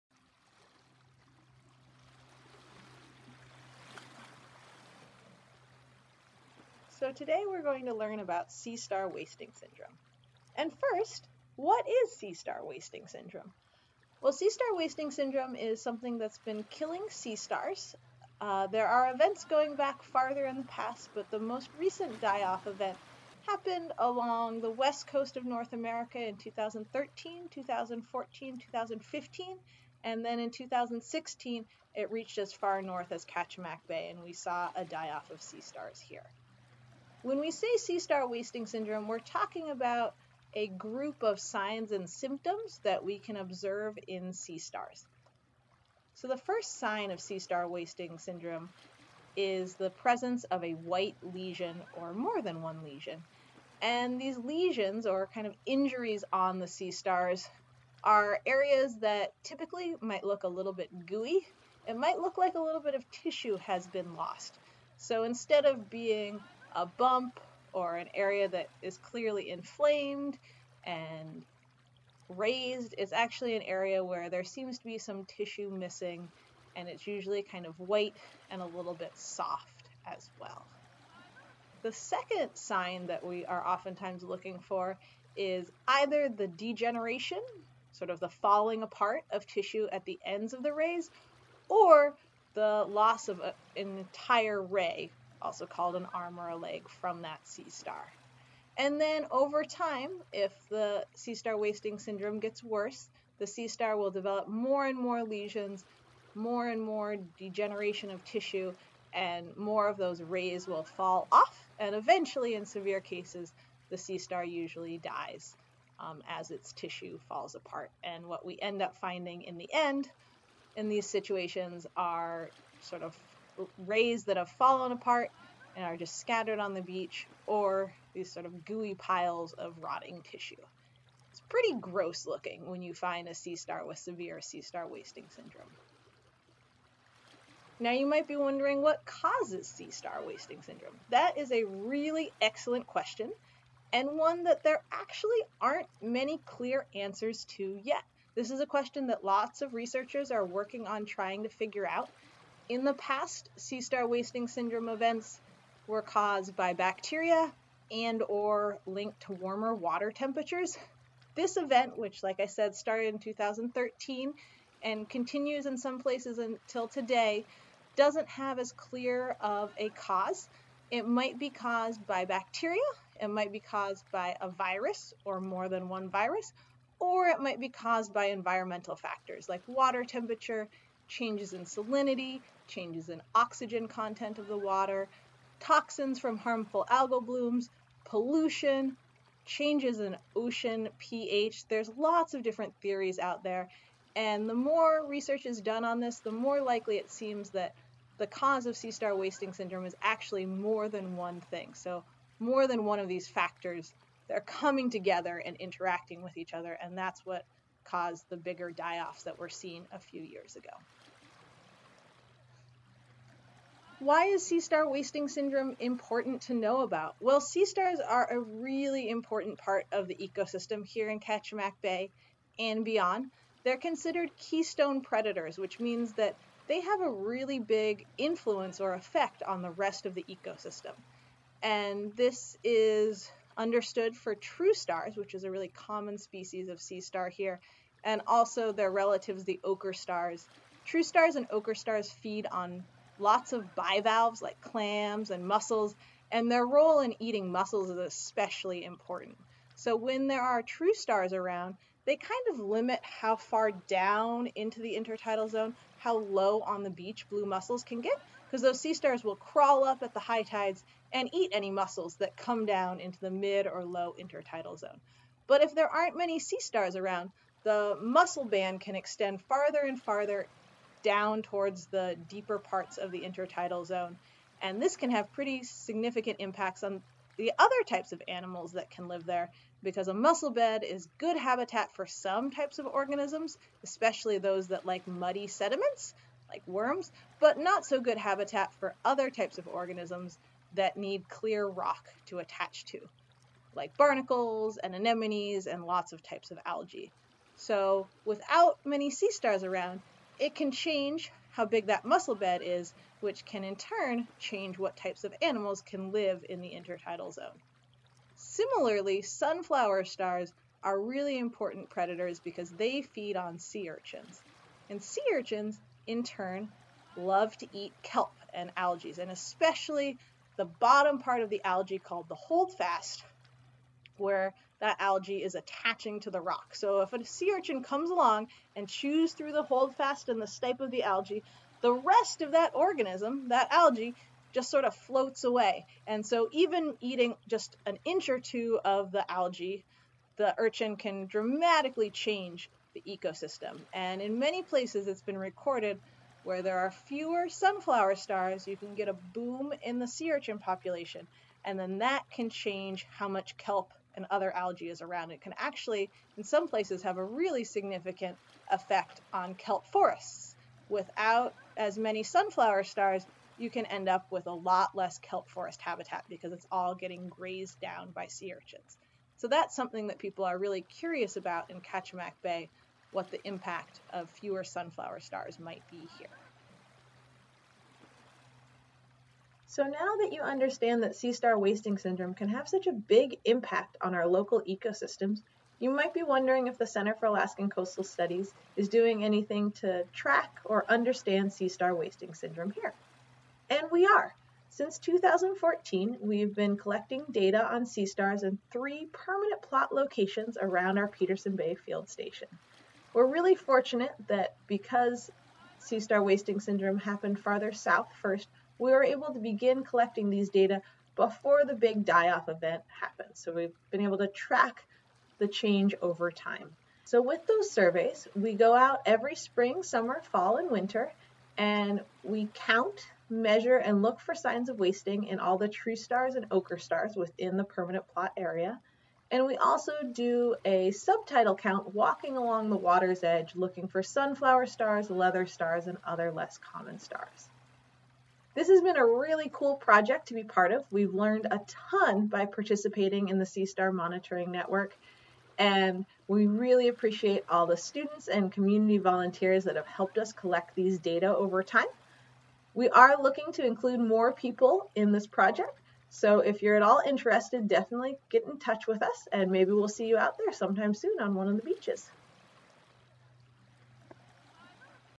An Introduction to Sea Star Wasting Syndrome
Learn about the mysterious die-off of sea stars across the West Coast of North America and how the effects are rippling through the ecosystem. This video provides a brief introduction to sea star wasting syndrome with the Center for Alaskan Coastal Studies. Here we answer 4 major questions:
What is sea star wasting syndrome?
What causes sea star wasting syndrome?
Why is sea star wasting syndrome important to know about?
and

How is the Center for Alaskan Coastal Studies helping to track and understand sea star wasting syndrome?

This video was made for upper elementary through college students.